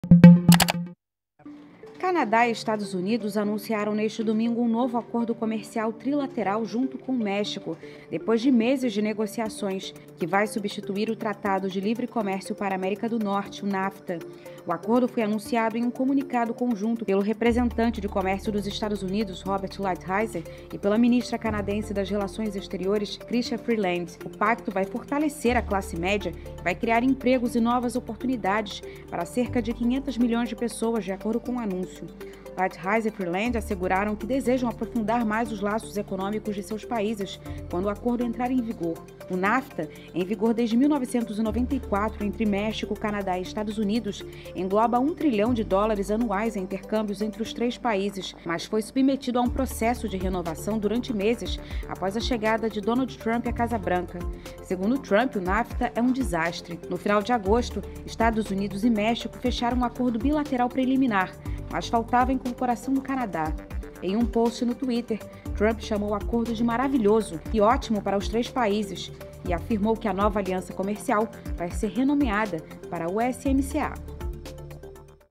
Canadá e EUA fecham acordo comercial com México em substituição ao Nafta
Washington, 30 set (EFE).- Canadá e Estados Unidos anunciaram neste domingo um novo acordo comercial trilateral junto com o México depois de meses de negociação, que substituirá o Tratado de Livre-Comércio para a América do Norte, o Nafta.

"Hoje, Canadá e Estados Unidos fecharam um acordo, junto com México, para um novo e modernizado acordo comercial para o século XXI: o acordo Estados Unidos-México-Canadá (USMCA, na sigla em inglês)", informaram em comunicado conjunto o representante de Comércio dos EUA, Robert Lighthizer, e a ministra de Exteriores canadense, Chrystia Freeland.

IMAGENS ARQUIVO.